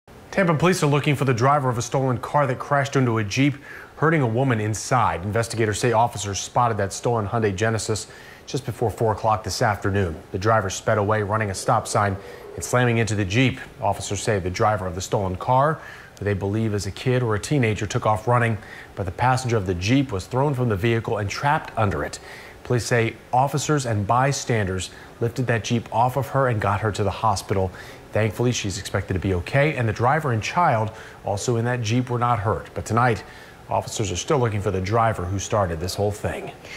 Suspect fled after he ran stop sign, crashed into vehicle in Tampa, police say
Tampa police are searching for a juvenile who they said stole a vehicle and ran away from the area after crashing into another vehicle Monday afternoon.